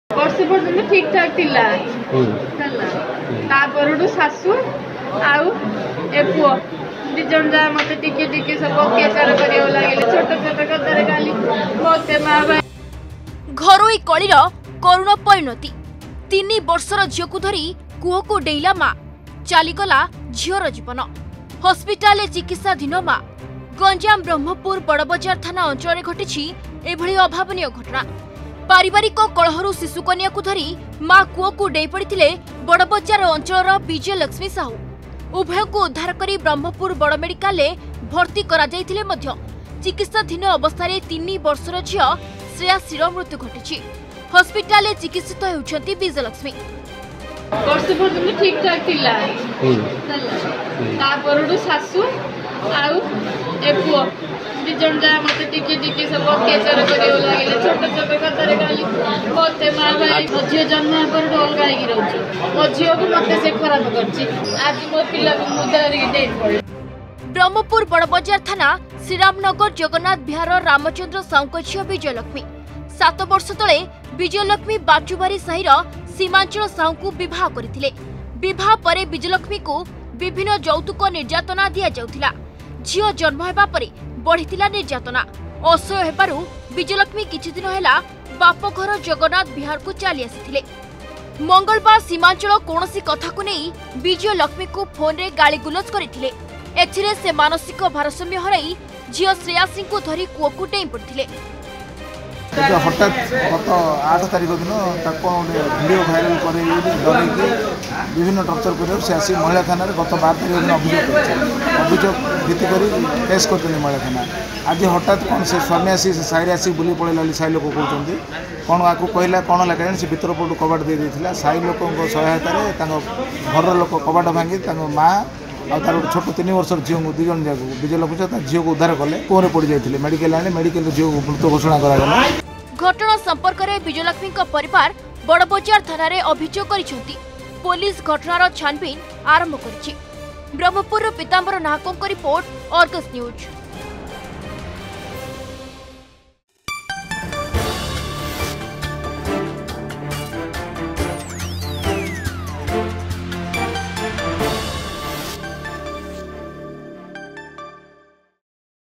ଶିଶୁକନ୍ୟାକୁ ଧରି କୂଅକୁ ଡେଇଁଲା ମା’
ଗଞ୍ଜାମ ବ୍ରହ୍ମପୁର ବଡବଜାର ଥାନା ଅଞ୍ଚଳରେ ଘଟିଛି ଏଭଳି ଅଭାବନୀୟ ଘଟଣା । ପାରିବାରିକ କଳହରୁ ଶିଶୁକନ୍ୟାକୁ ଧରି ମା’ କୂଅକୁ ଡେଇଁ ପଡିଥିଲେ ବଡ଼ବଜାର ଅଂଚଳର ବିଜୟଲକ୍ଷ୍ମୀ ସାହୁ.